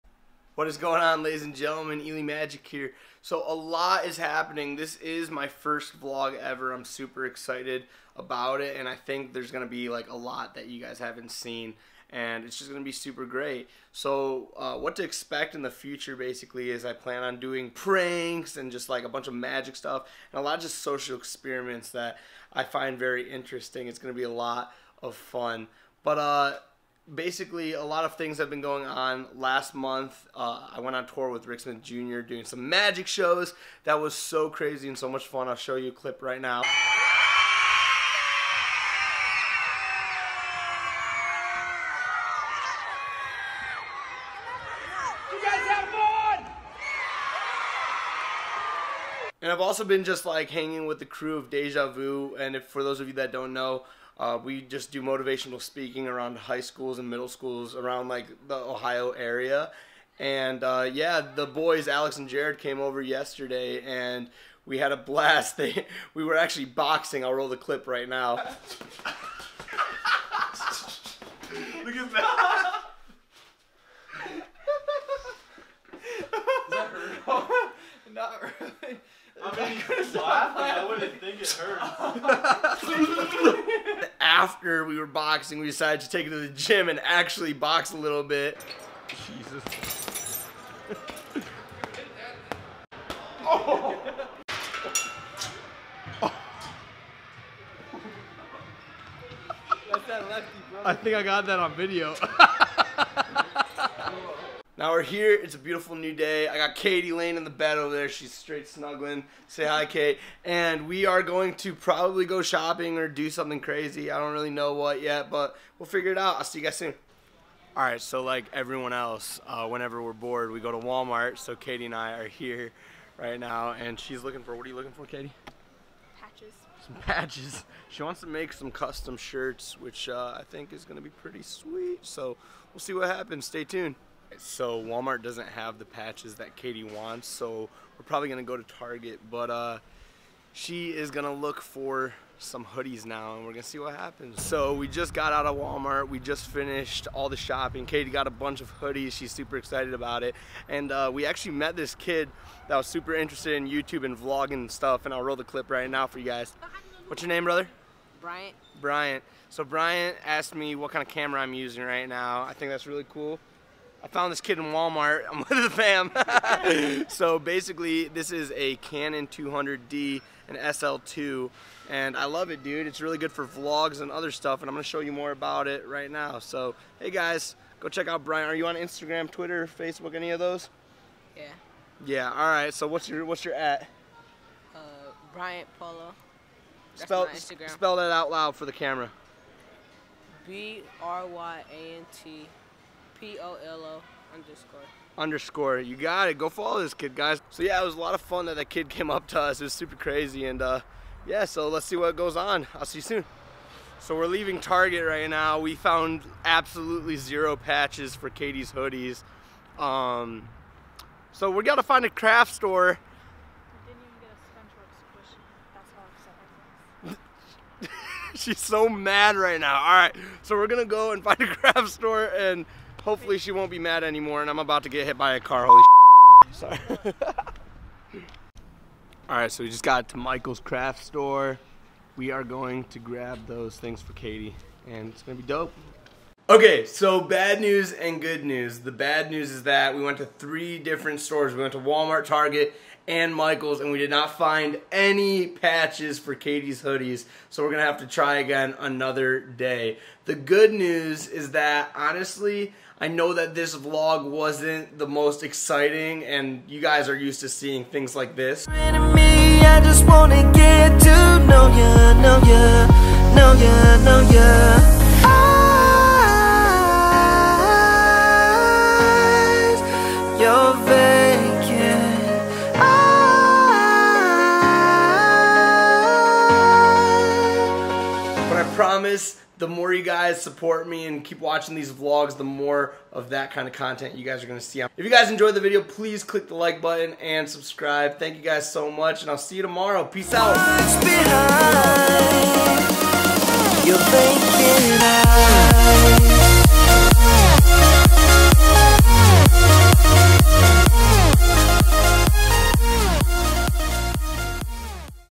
FIRST VLOG EVER | Elie Magic
I’m a 19 year old Magician living in Cleveland. This is my life.